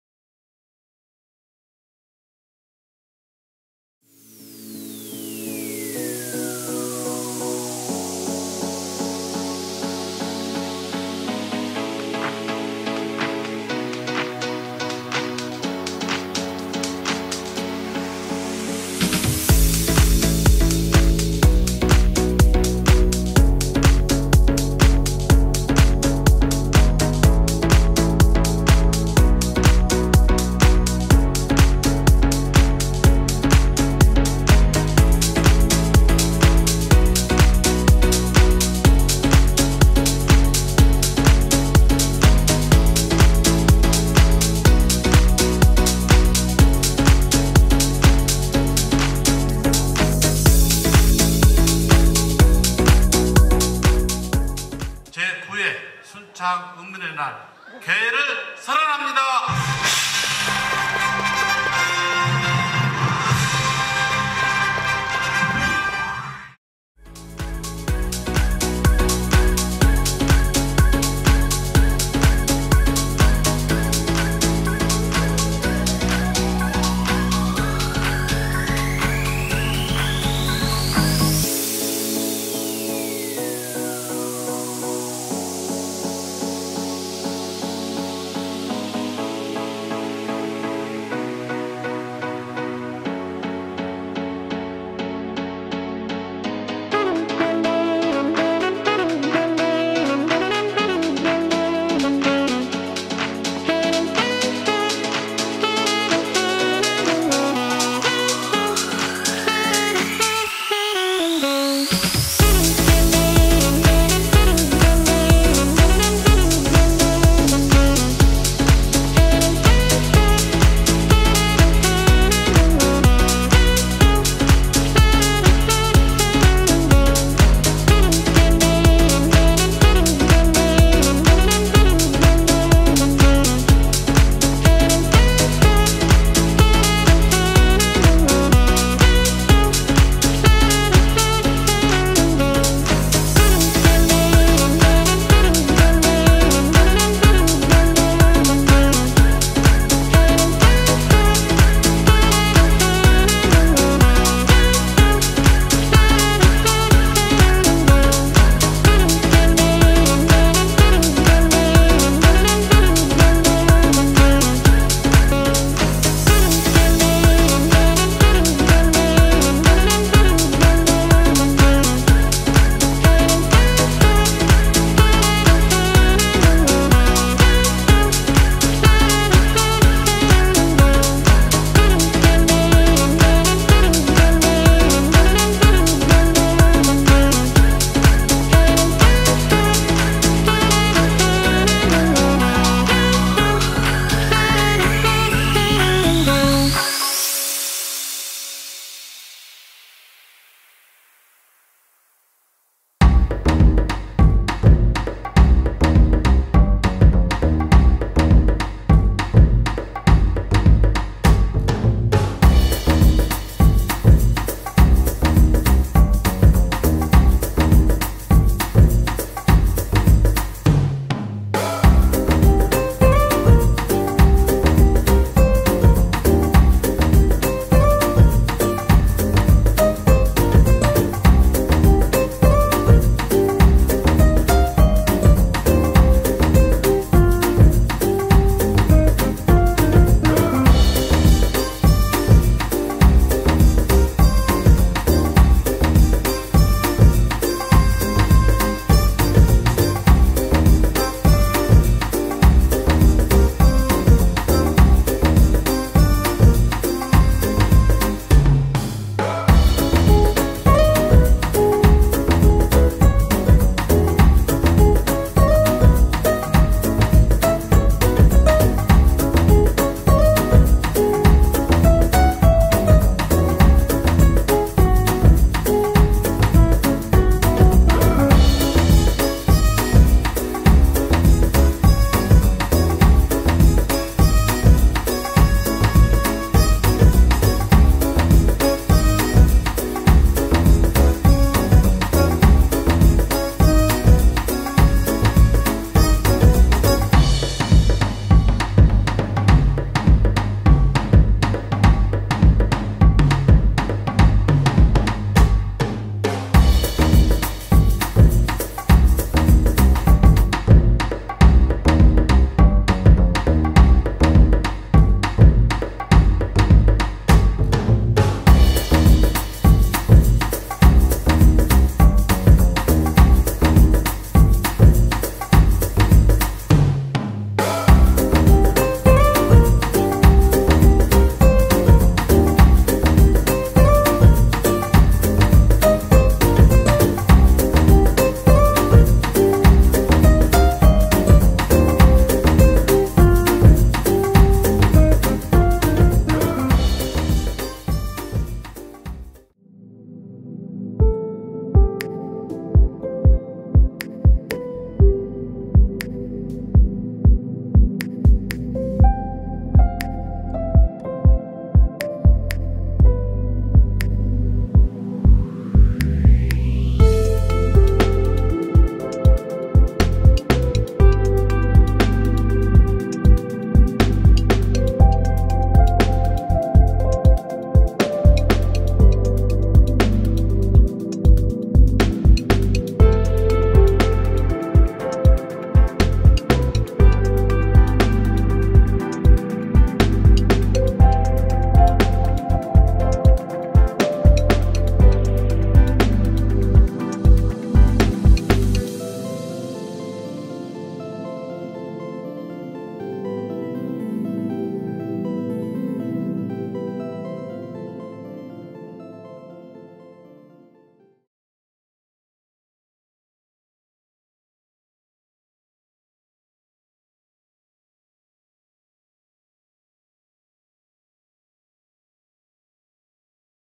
체험과 먹거리 가득한! 제9회 순창읍민의 날
코로나19로 인해 2년 동안 개최되지 못했던 읍민의 날 행사가
10월 8일 일품공원 일원에서 열렸습니다.